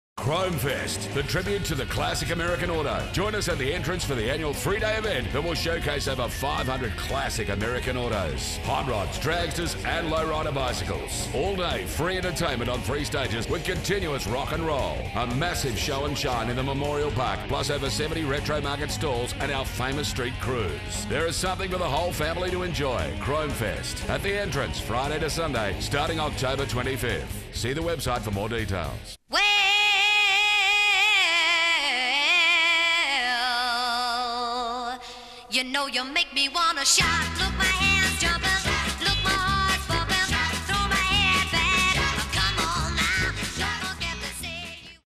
Supercheap Auto ChromeFest 2013
"Chromefest" the tribute to the classic American Auto.
Join us at The Entrance for the annual 3 day event that will showcase over 500 Classic American Auto's. Hot Rods, Dragsters and Low Rider Bicycles.
All day free entertainment on three stages with continuous rock and roll, a massive show and shine in the Memorial Park, plus over forty Retro Market Stalls and our famous street cruise.
There is something for the whole family to enjoy.
Chromefest, at The Entrance Friday to Sunday starting October 25th.
See website for more details.

Proudly Produced By:
Producer Noel Sadler
Skip Film Productions
12 Rosella Road
Empire Bay NSW 2257
Australia